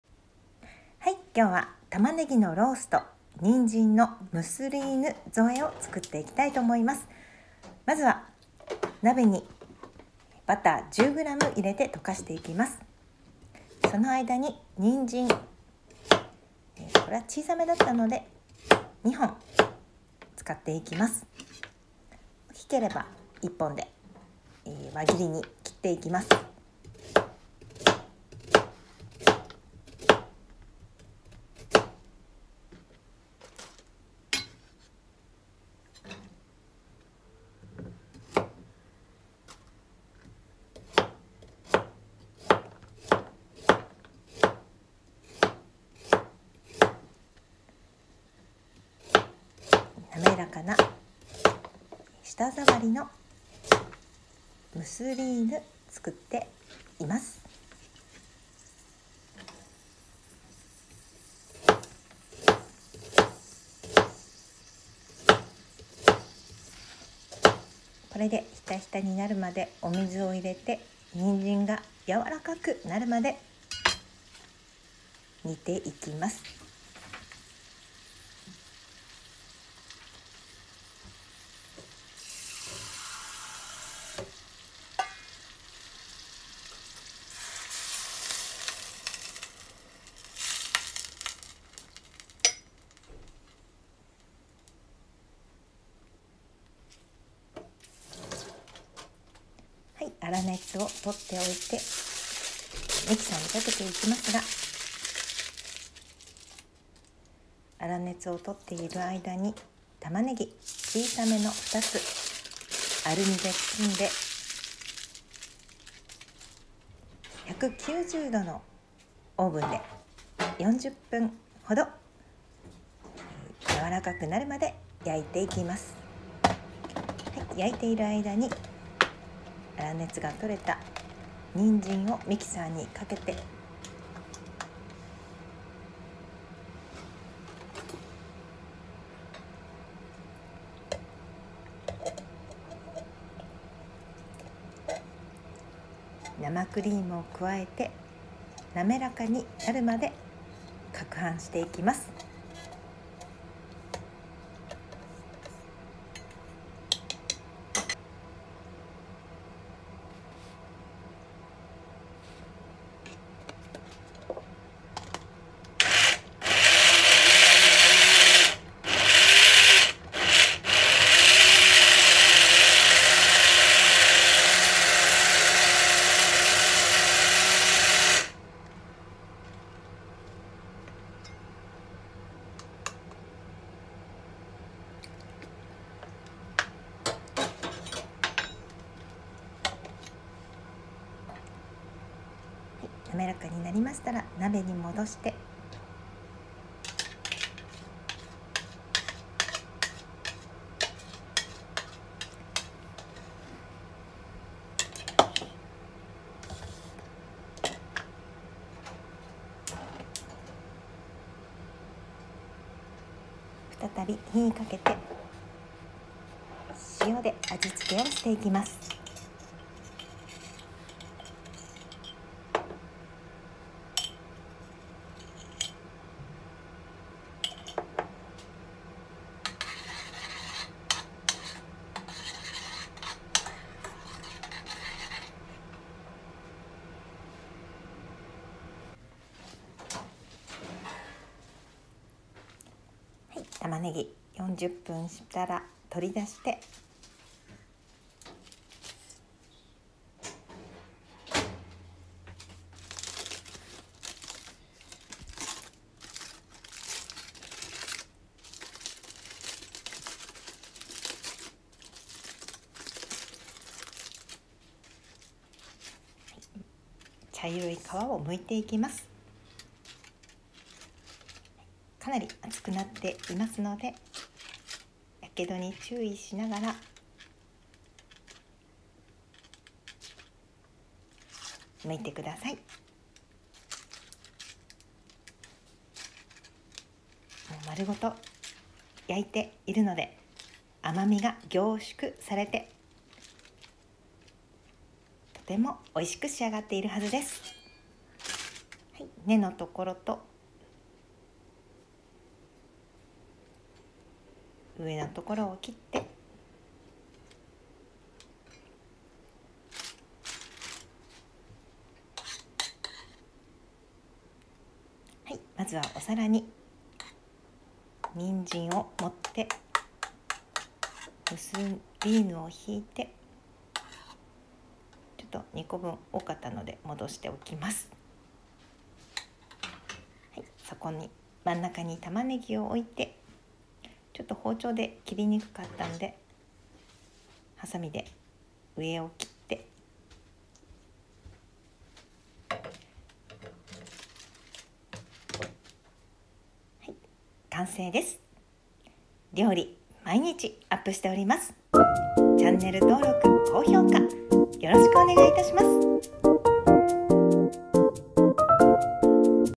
玉ねぎ丸々入れてニンジンのソースで頂きました。
味は濃い目になっております音声による解説付きです。